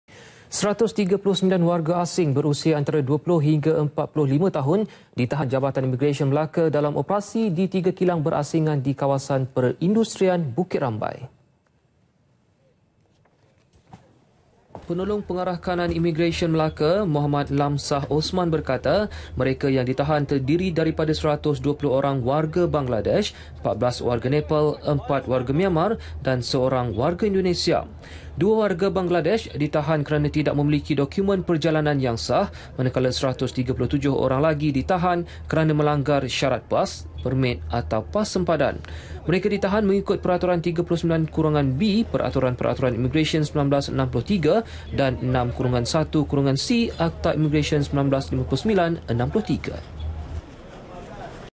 PATI- 139 WARGA ASING DITAHAN DI MELAKA [24 OKT 2015]
139 warga asing berusia antara 20 hingga 45 tahun ditahan Jabatan Imigresen Melaka dalam operasi di tiga kilang berasingan di Kawasan Perindustrian Bukit Rambai.